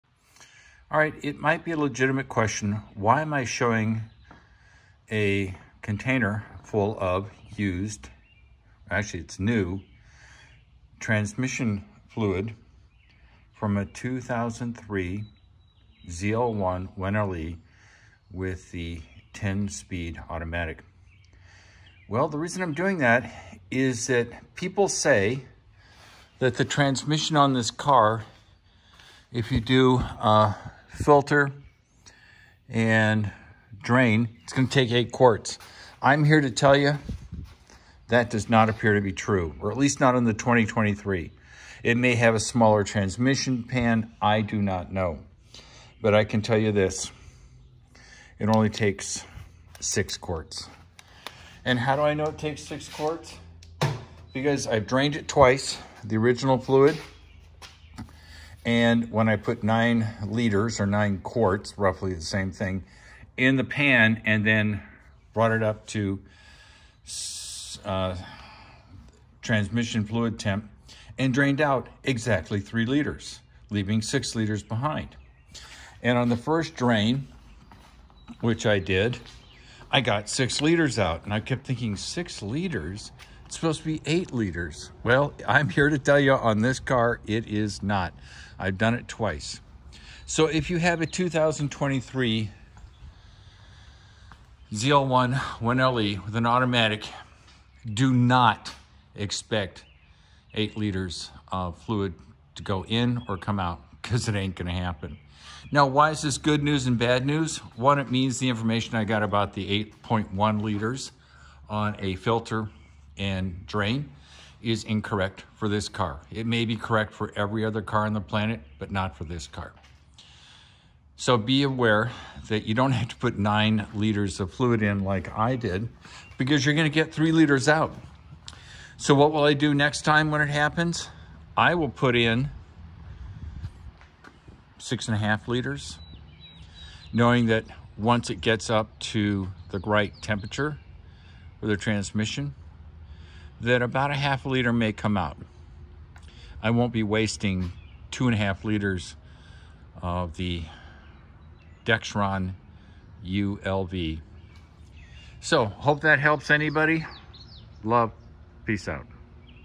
6 liters not 8.1 liters in a 2023 Camaro ZL1 1LE 10L90 A10 automatic.
6 liters not 8.1 liters in a 2023 Camaro ZL1 1LE 10L90 A10 automatic.